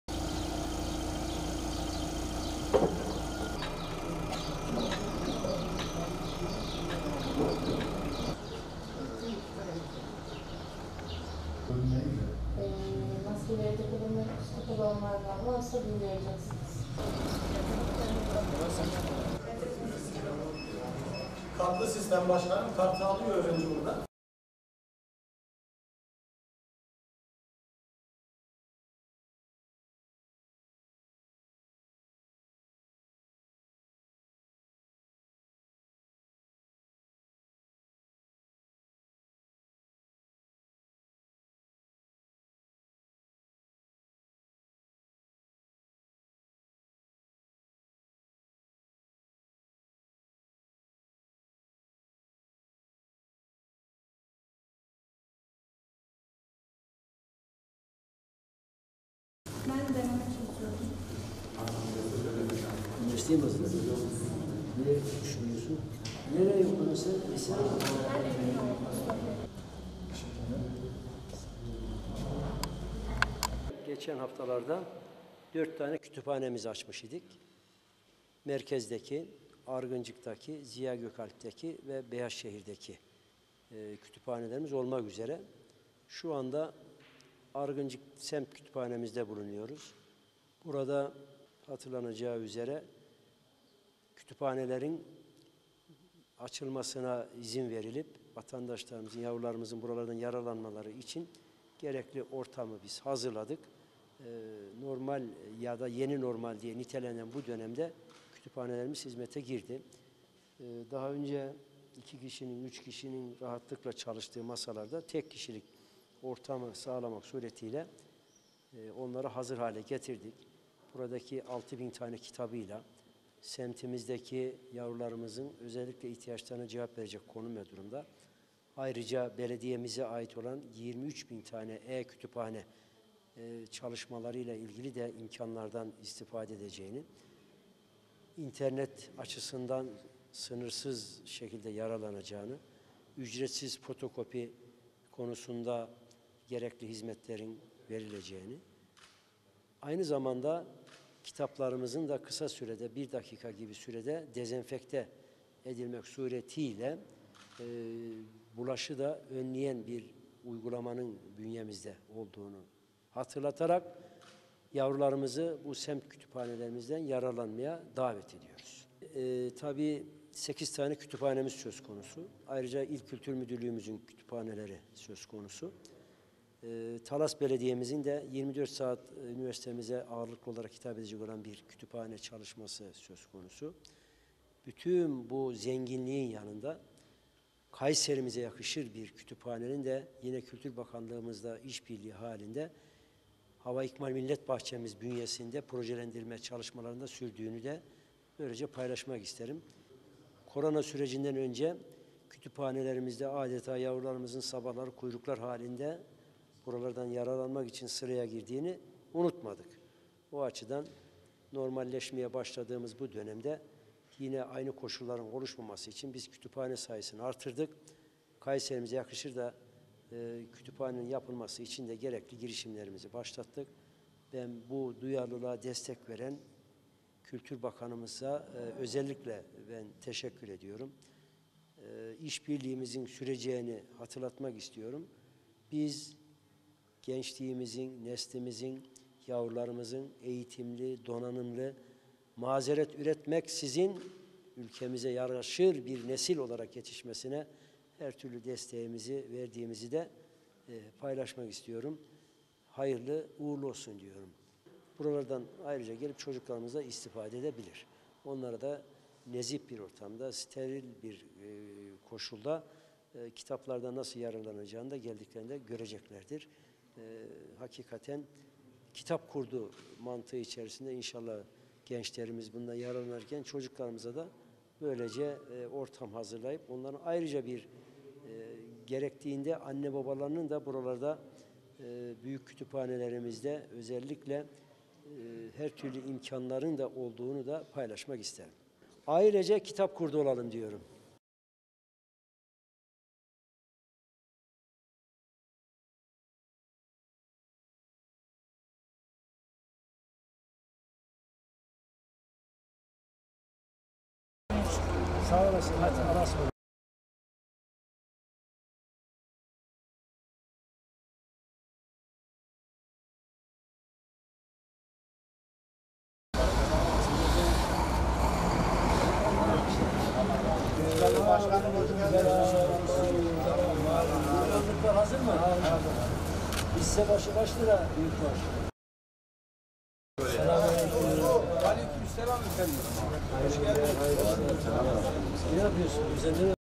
Başkan Büyükkılıç Argıncık Semt Kütüphanesini Gezdi
KAYSERİ (MHA) - Kayseri Büyükşehir Belediye Başkanı Dr. Memduh Büyükkılıç, geçtiğimiz haftalarda açılan Argıncık Semt Kütüphanesi’ni ziyaret etti.

Kayseri Büyükşehir Belediye Başkanı Memduh Büyükkılıç, geçen haftalarda açılan Beyazşehir Halk Kütüphanesi, Ziyagökalp ve Argıncık Semt Kütüphaneleri’nin açılış törenine katılmıştı. Yeni normal hayatta tedbir ve kurallara uyulmasına çok önem veren Başkan Büyükkılıç, haziran itibariyle hizmet vermeye başlayan kütüphanelerden biri olan Argıncık Semt Kütüphanesi’ni gezdi ve vatandaşların daha hijyenik ve steril bir ortamda olmaları için tüm kontrolleri sağladı. Kütüphaneye ders çalışmak ve kitap okumak için gelen öğrencilerle de sohbet eden Başkan Büyükkılıç, “ Yavrularımızın kütüphanelerimizden yararlanmaları için gerekli ortamı hazırladık.  Yeni normal diye nitelendirdiğimiz bu dönemde kütüphanelerimiz hizmete girdi.  Daha önce üç kişinin rahatlıkla çalıştığı masalarda tek kişi çalışır pozisyona getirdik. Argıncık Semt Kütüphanemizi 6 bin kitabımızla semtimizdeki yavrularımızın ihtiyaçlarına cevap verecek şekilde ayarladık. Gençlerimizin ayrıca belediyemize ait 23 bin e- kütüphane çalışmalarıyla da ilgili istifade edeceğini, internet açısından sınırsız şekilde yaralanacağını, ücretsiz fotokopi konusunda gerekli hizmetlerin verileceğine ve kitap sterilizasyon cihazı ile de kullanıcılar bütün kitapları steril hale getirerek kullanabilecekler. Bunu da hatırlatarak yavrularımızı semt kütüphanelerimizden yararlanmaya davet ediyoruz” diye konuştu.

“KAYSERİ’YE YAKIŞIR BİR KÜTÜPHANE DAHA OLACAK”

Kayseri’nin dev yatırımı olan Hava İkmal Millet Bahçesi’nin içerisinde de Halk Kütüphanesi açılacağını dile getiren Başkan Büyükkılıç, şöyle konuştu; Bütün bu zenginliğin yanında Kayseri’mize yakışır bir kütüphanenin de Kültür Bakanlığımızın iş birliği ile Hava İkmal Millet Bahçesi’nde açacağımızı söylemek isterim. Corona süresi öncesinde yavrularımızın kütüphanelerin önünde sıraya girdiğini unutmadık. Normalleşmeye başladığımız şu süreçte yine aynı koşullar oluşmaması için biz kütüphane sayımızı artırdık. Hava İkmal Millet Bahçe’mizin içerisine de Kayseri’mize yakışır bir şekilde kütüphane yapılması için gerekli girişimlerimizi başlattık. Bu duyarlılığa destek veren Kültür Bakanımıza teşekkür ediyorum. Biz gençlerimizin eğitimli, donanımlı, mazeret üretmeksizin ülkemize yaraşır bir nesil olarak yetişmesi için her türlü desteğimizi verdiğimizi paylaşmak istiyorum. Hayırlı uğurlu olsun diliyorum.

“AİLECEK KİTAP KURDU OLALIM”

Şehre yapılan bütün kütüphanelerde çocuk kütüphanesi olduğunu ve bundan sonrada açılacak olan her kütüphanede olacağını söyleyen Büyükşehir Belediye Başkanı Memduh Büyükkılıç, “Nezih bir ortamda steril bir koşulda, kitap yurdu mantığı içerisinde inşallah çocuklarımıza ayrıca ortam hazırlandı. Anne babalar içinde çocukları buradayken her türlü imkanımız mevcut. Ailecek kitap kurdu olalım diyorum ve sizleri kütüphanelerimize davet ediyorum” dedi.

Argıncık Semt Kütüphanesi’ni gezdikten sonra Argıncık Mahalle Muhtarlığı’na ziyarette bulunan Başkan Büyükkılıç, daha sonra da Argıncık Esnafını dolaşarak hayırlı işler temennisinde bulundu.